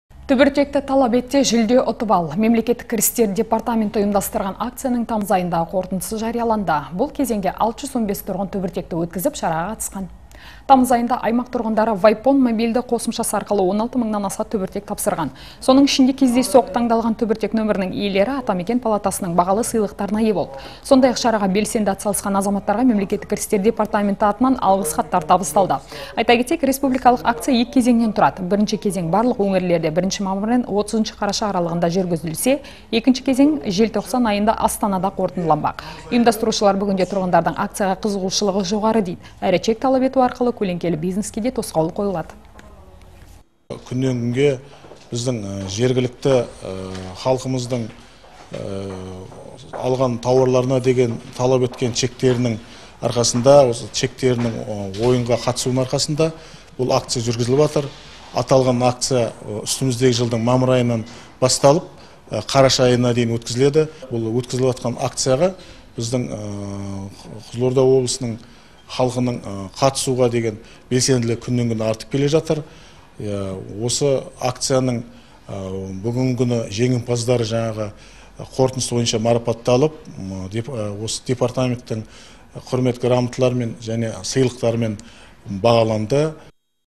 Акция жүлдегерлері анықталды
Түбіртекті талап ет те, жүлде ұтып ал! Мемлекеттік кірістер департаменті ұйымдастырған акцияның тамыз айындағы қорытындысы жарияланды. Бұл кезеңде 615 тұрғын түбіртек өткізіп, шараға қатысқан.